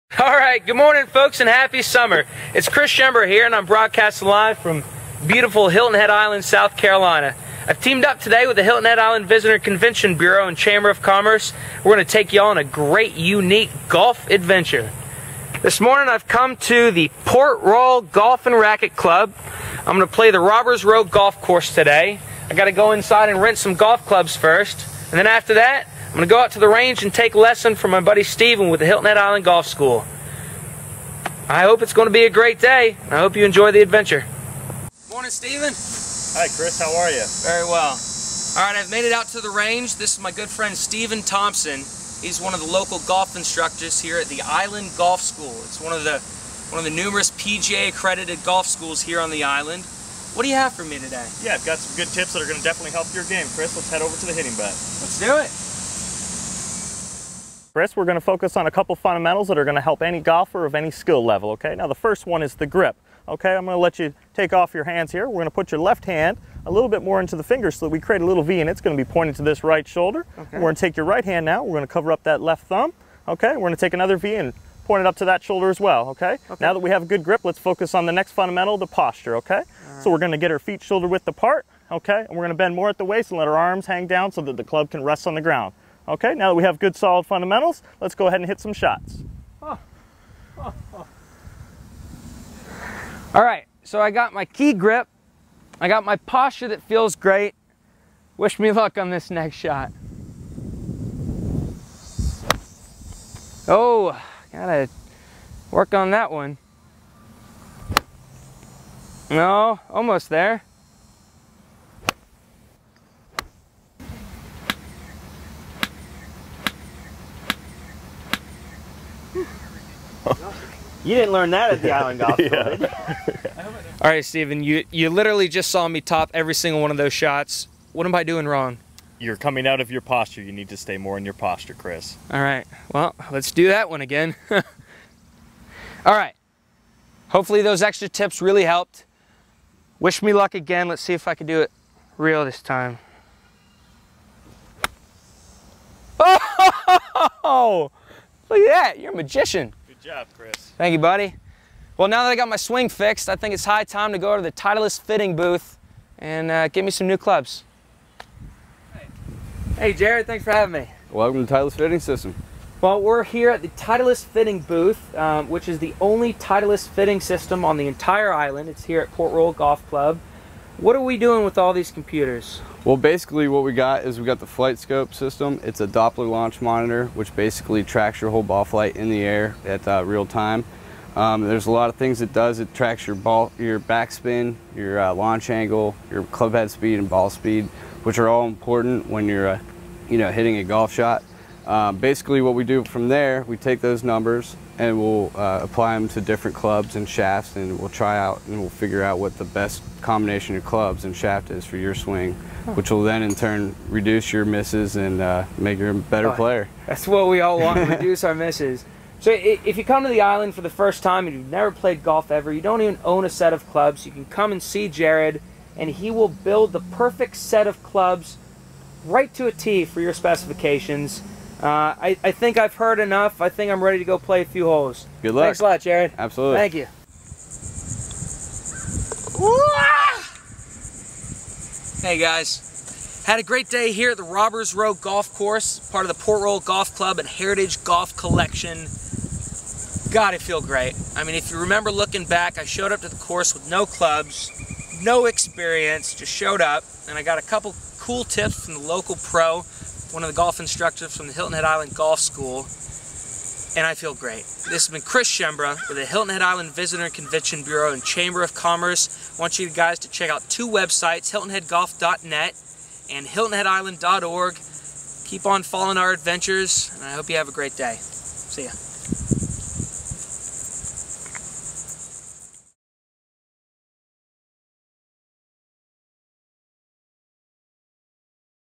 Hilton Head Island Golf Adventure with Chris - Courtesy Hilton Head Island Visitor & Conv. Bureau
Local Islander Chris Schembra, working for the HIlton Head Island Visitor & Convention Bureau, takes you on a golf adventure to Port Royal Golf Club, where he takes a golf lesson, tries out some new golf clubs, and plays a round of golf at Robbers Row.  Chris shows that golfing can be fun and not as intimidating as you would expect.  Learns some tips on golf posture and how not to tip the ball.